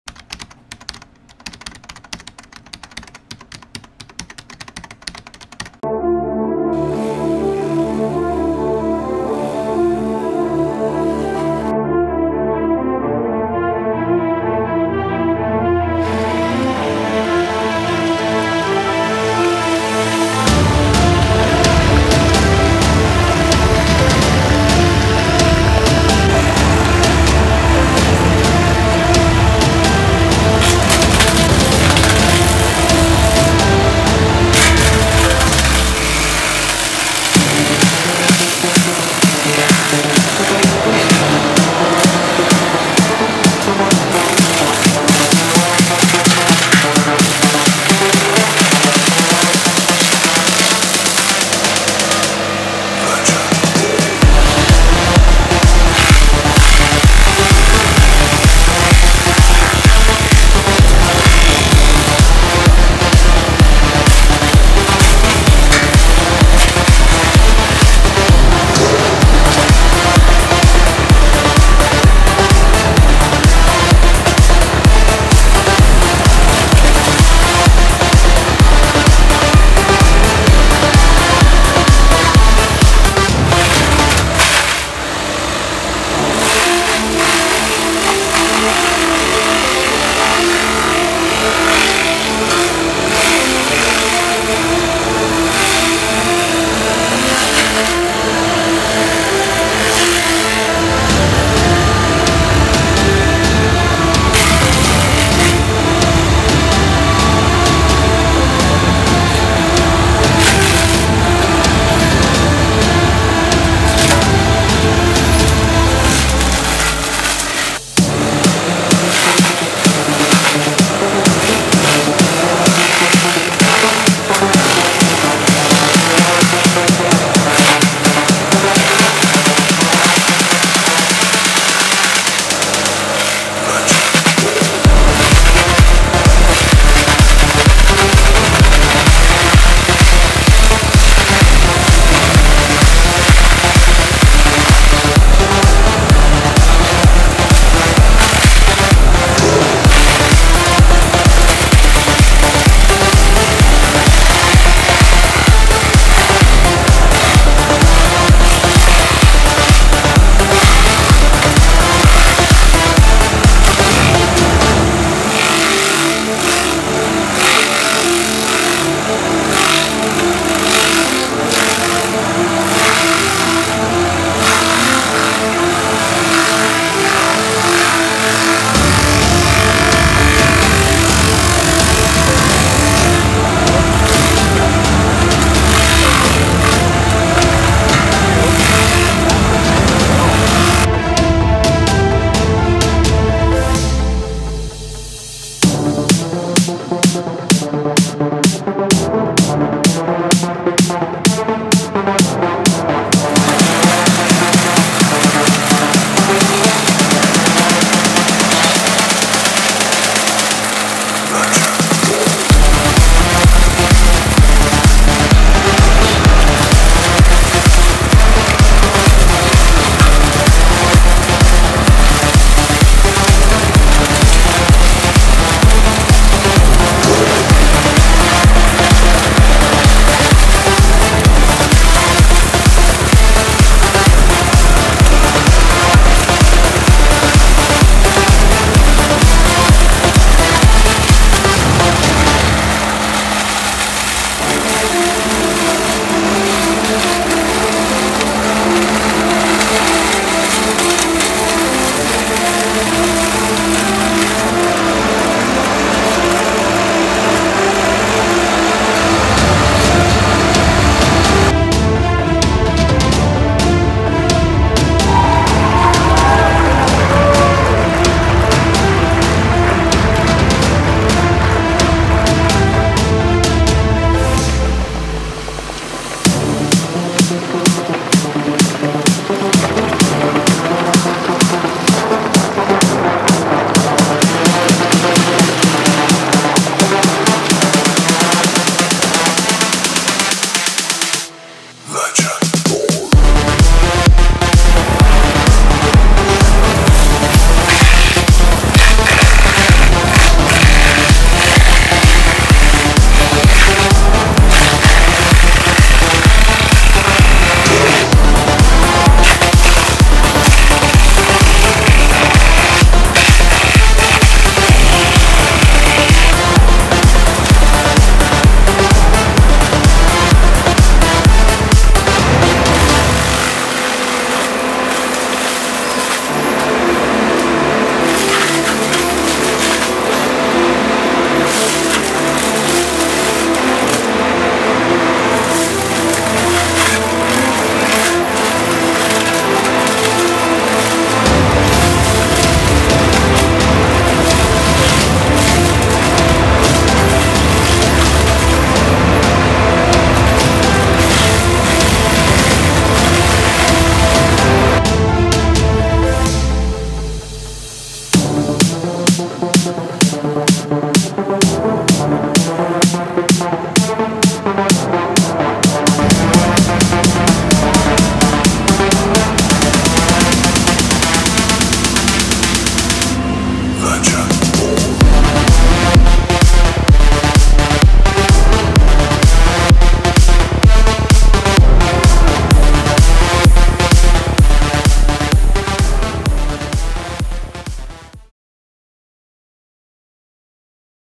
FAE碎木機配件開箱實作
來自義大利品牌配件*FAE碎木機配件*

BLO/EX50碎木機
適用:2-4頓用

DML/HY-100碎木機
 適用:5-13頓